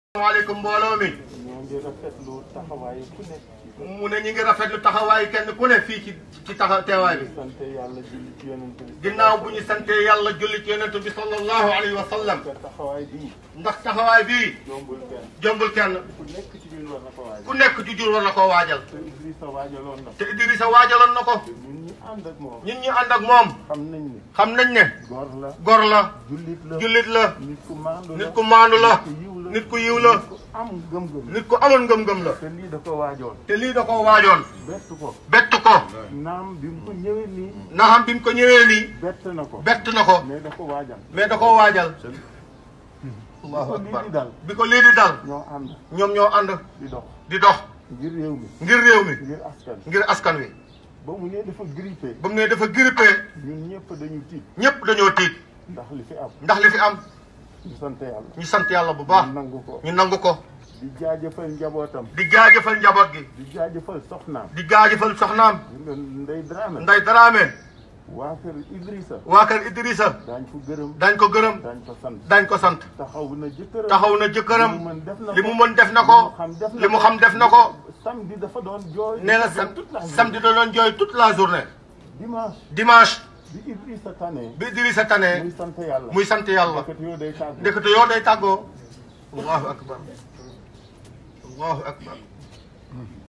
Levée du corps d'Idrissa Diallo: Le témoignage émouvant de Khalifa Sall qui perd un lieutenant
Le Grand Maire Idrissa Diallo raconté par le Président Khalifa Ababacar Sall